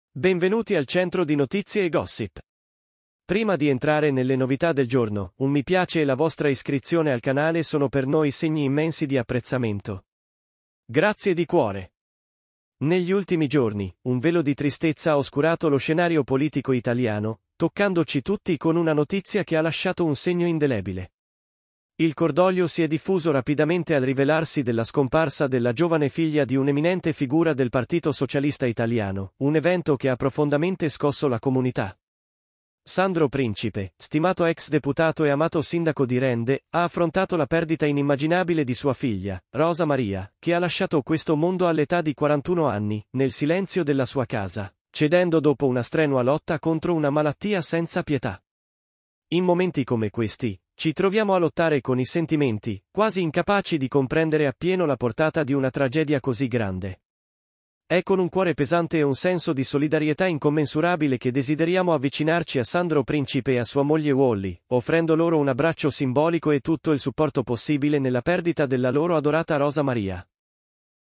POLITICA ITALIANA IN LUTTO,È MORTA POCHI MINUTI FA ERA GIOVANISSIMA
Il testo racconta la commovente storia di Rosa Maria Principe, la giovane figlia dell'ex deputato e sindaco di Rende, Sandro Principe, esponente del Partito Socialista Italiano, la cui vita è stata tragicamente interrotta da una malattia. La sua scomparsa ha scatenato un'ondata di cordoglio e solidarietà nel panorama politico italiano e nella comunità di Rende, evidenziando la profondità della perdita sentita da tutti coloro che la conoscevano e ammiravano.